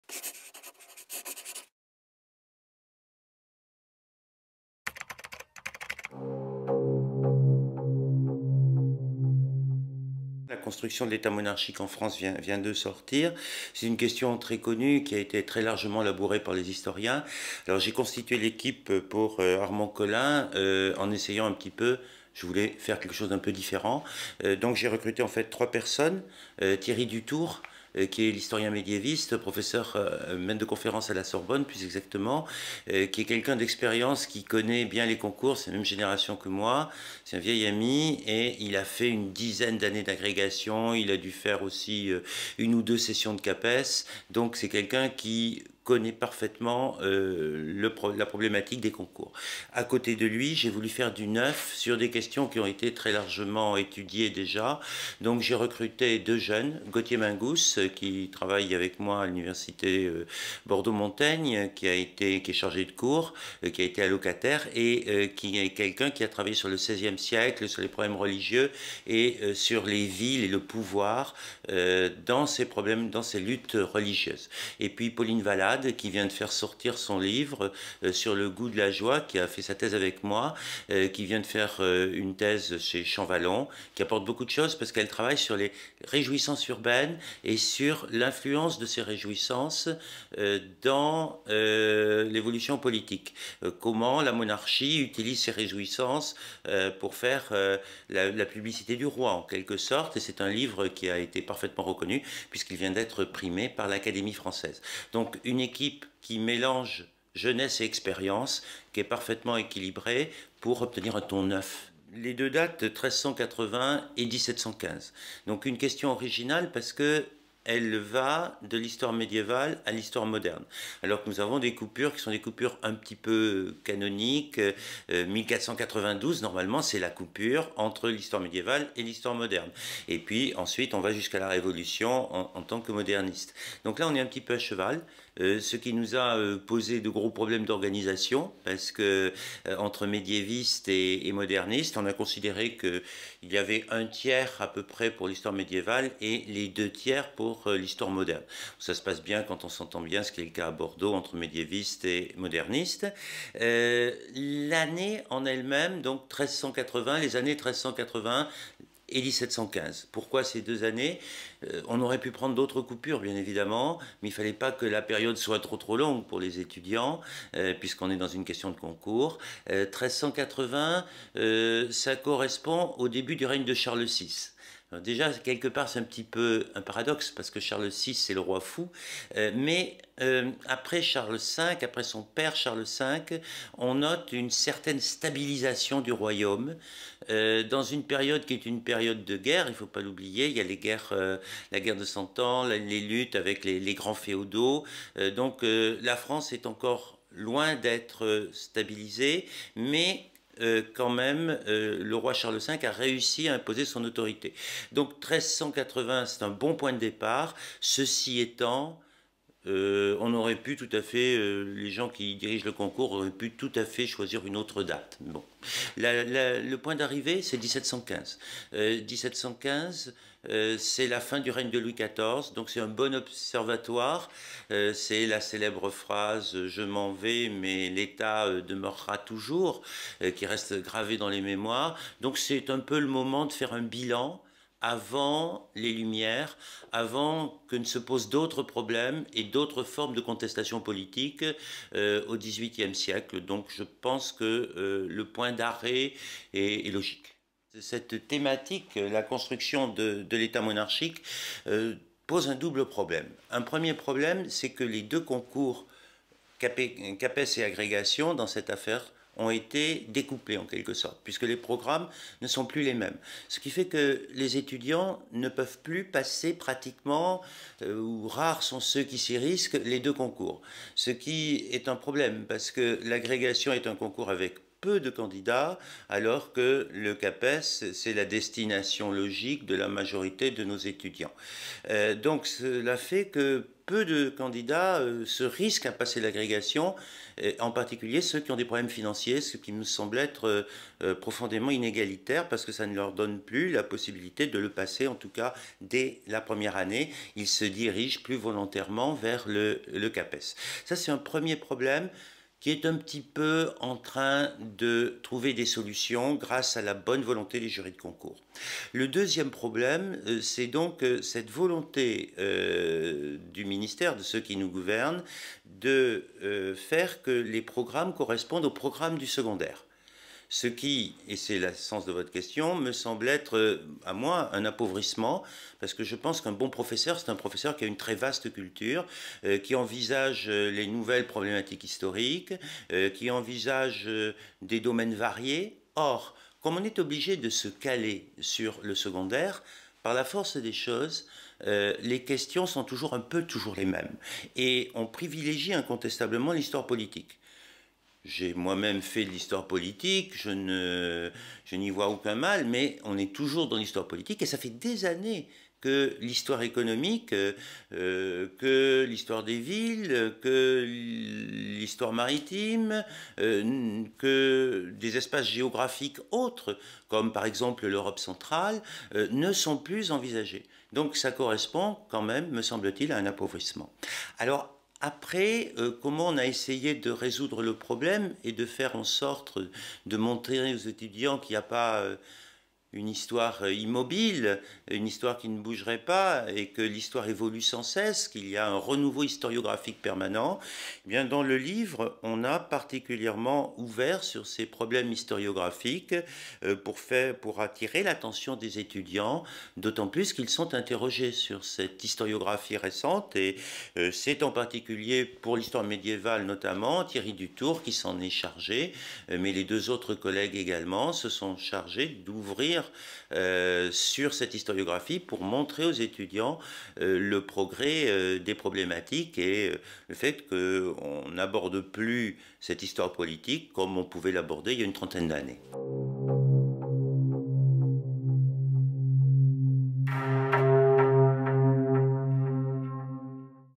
Michel Figeac - La construction de l'Etat monarchique en France de 1380 à 1715
Michel Figeac vous présente son ouvrage "La construction de l'Etat monarchique en France de 1380 à 1715" aux éditions Armand Colin.